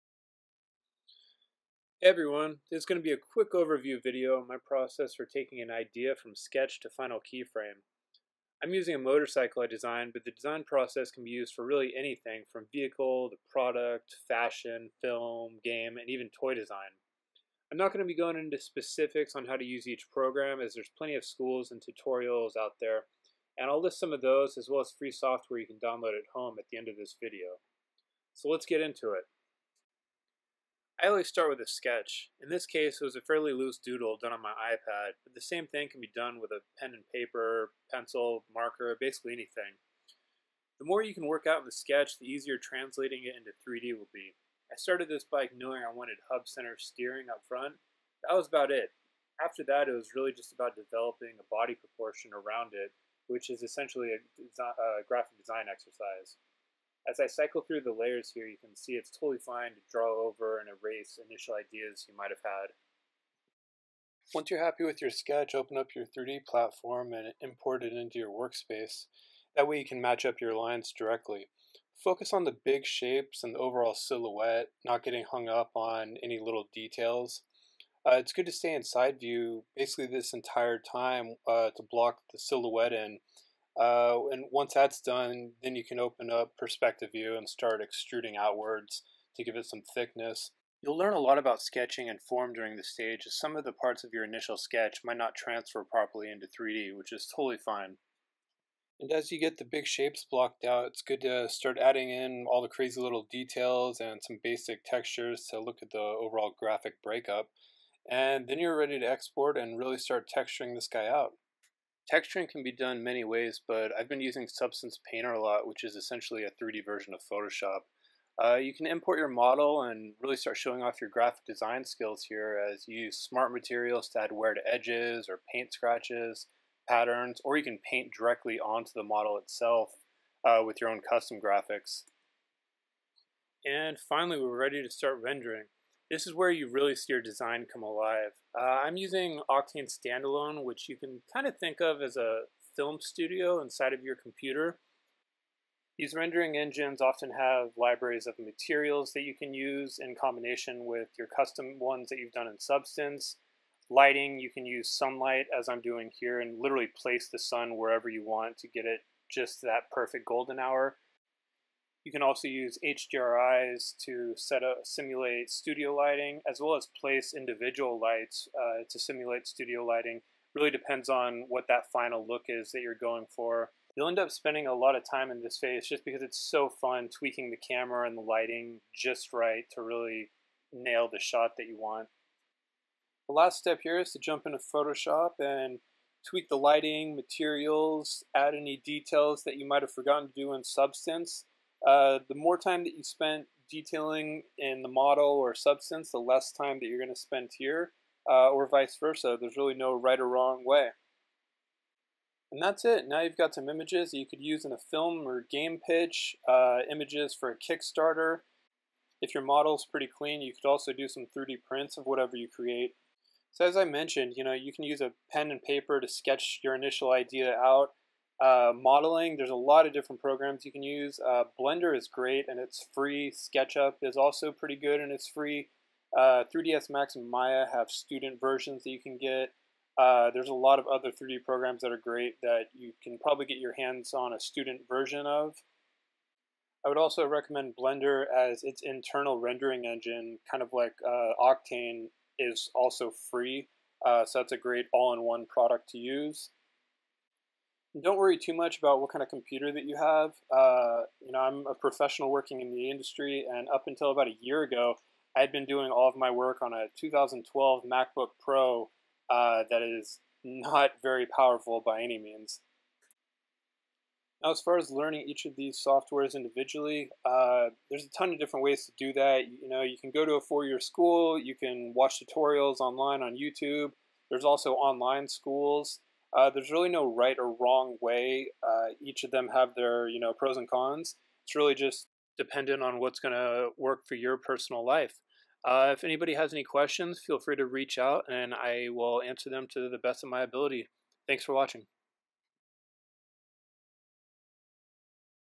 Concept Design an electric motorcycle by Shane Baxley
Concept Design overview in 5 minutes.